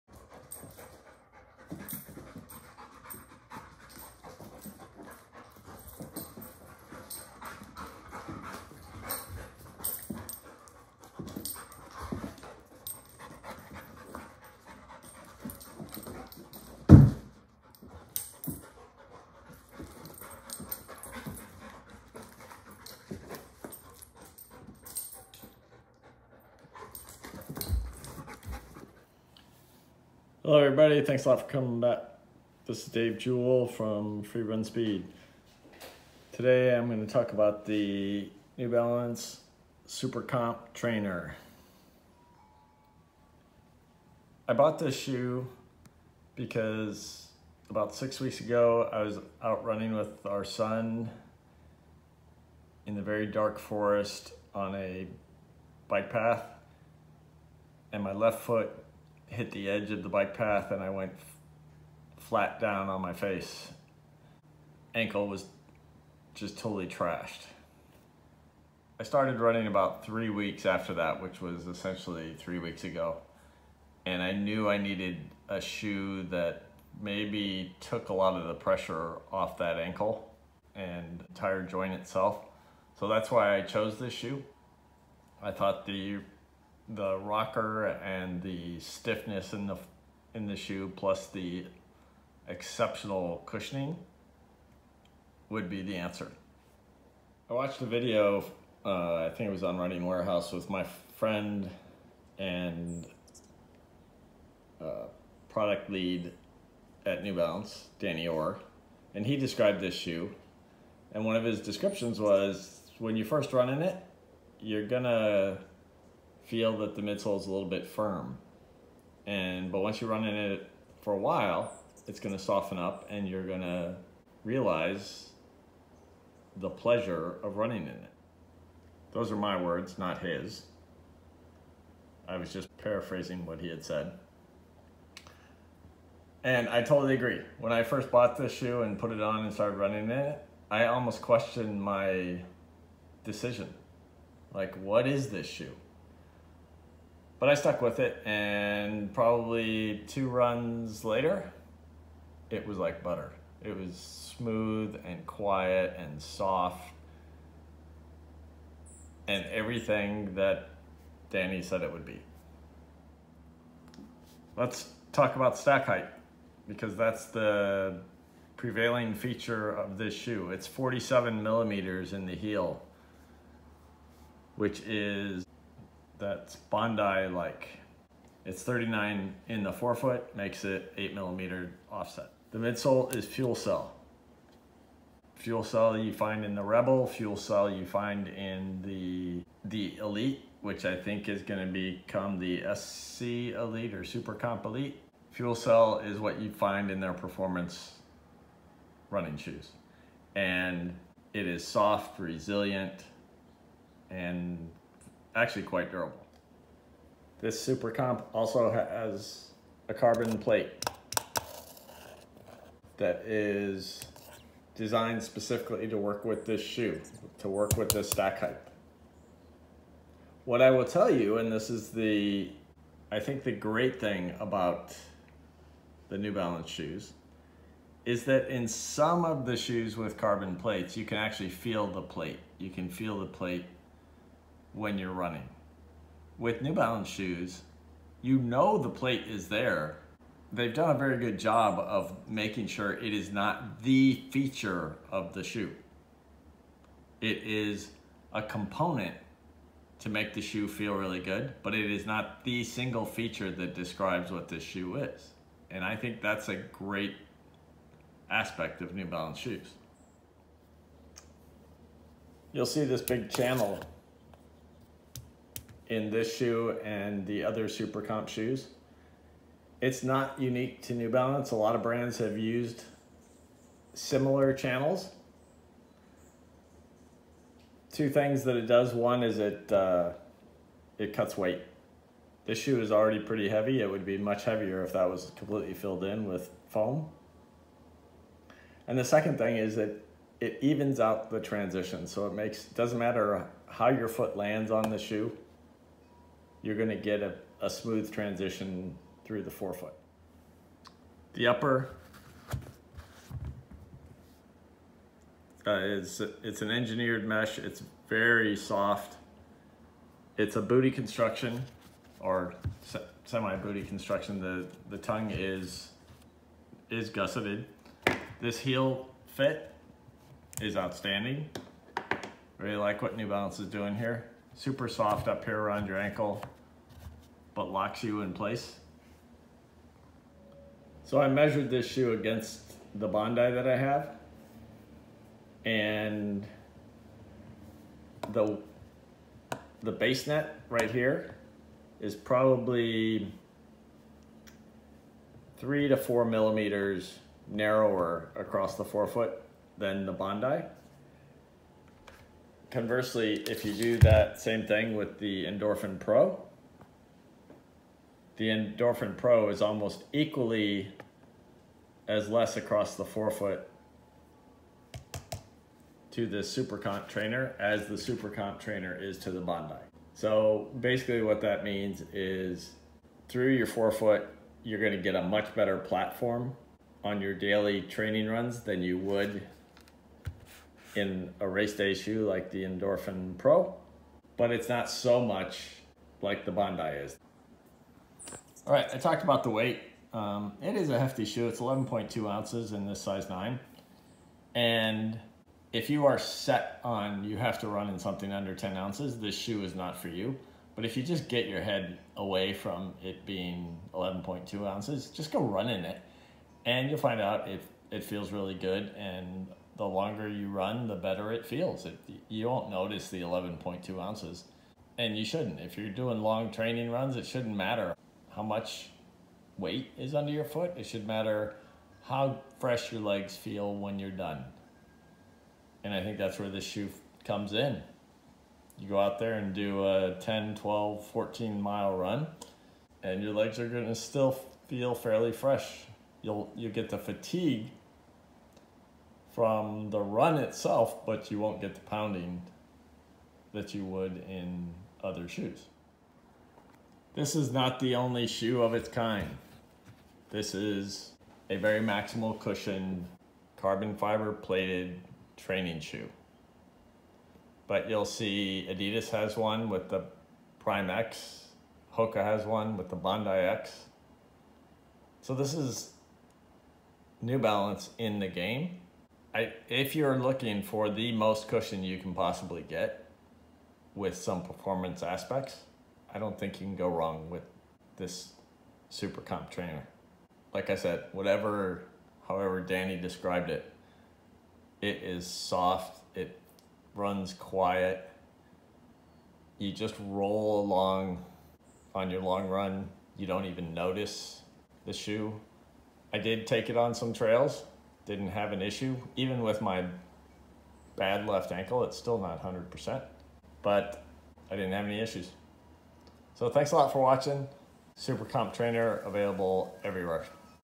New Balance Super Comp Trainer
Over stacked super shoes are becoming a thing. Hoka Bondi X, adidas adizero Prime X and now the New Balance Super Comp Trainer. They all have in common a stack height well above the legal IAAF limit for Elite racing. If you are racing for money you can’t race in them. If you are simply trying to train for and run a marathon for pleasure than these shoes are a viable option.